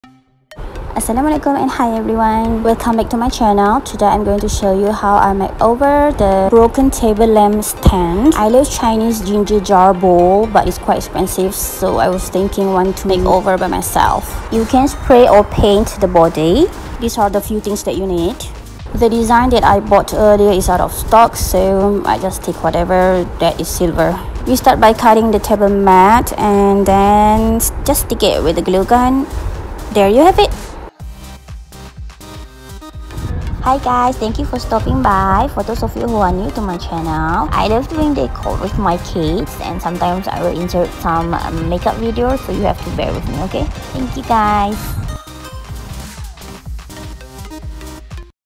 Eco Shop Haul Repurpose Broken Table Lamp into Ginger Jar
🍓If u happened to have a broken table lamp, do not throw it😅. We can do some makeover with it💃🏼.

I luv chinese ginger jar decor but its quite pricy. When i looked at this broken lamp stand..the idea pop up to me.

Im using Eco Shop table mat which is super cheap. Just cut and wrap around the body of the broken stand. Btw i forgot to mention that im using drawer knob as a decorative items to put on top of the stand.

Its pretty quick and easy guys.